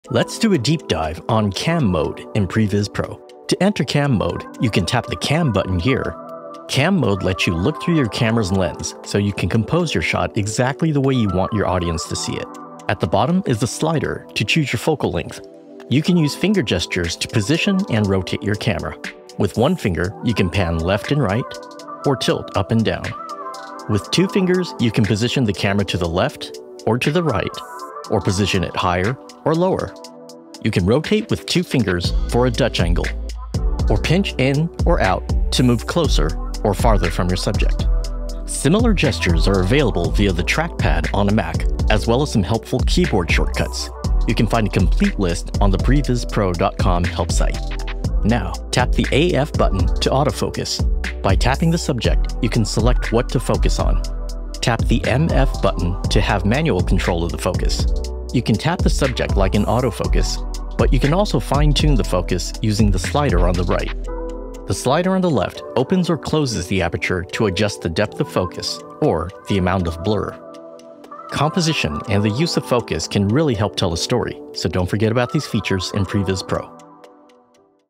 CAM Mode in Previs Pro
We're taking you deep into Previs Pro CAM Mode. CAM Mode lets you look through your camera lens to see and edit the perfect shot. Auto Focus. Manual Focus. Fine Tune. The choice is yours.